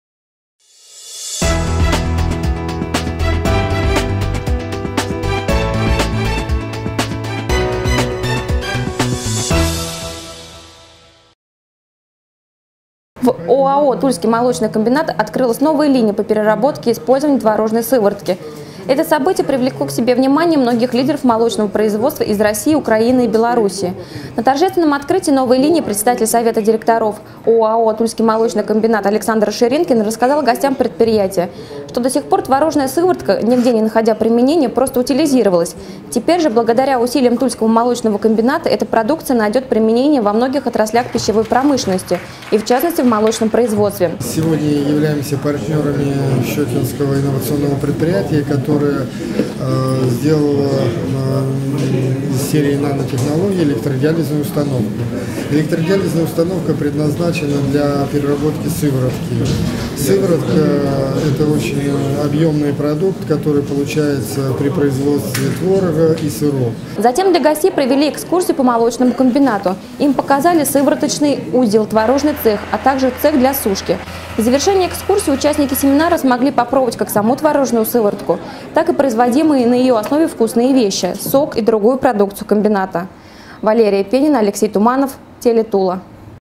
Из творожной сыворотки делают вкуснятину
В ОАО «Тульский молочный комбинат» открылась новая линия по переработке и использованию творожной сыворотки. Это событие привлекло к себе внимание многих лидеров молочного производства из России, Украины и Беларуси.
На торжественном открытии новой линии председатель совета директоров ОАО «Тульский молочный комбинат» Александр Ширинкин рассказал гостям предприятия, что до сих пор творожная сыворотка, нигде не находя применения, просто утилизировалась. Теперь же, благодаря усилиям Тульского молочного комбината, эта продукция найдет применение во многих отраслях пищевой промышленности, и в частности, в молочном производстве.
Затем для гостей провели экскурсию по молочному комбинату. Им показали сывороточный узел, творожный цех, а также цех для сушки.  В завершение экскурсии участники семинара смогли попробовать как саму творожную сыворотку, так и производимые на ее основе вкусные вещи: сок и другую продукцию комбината.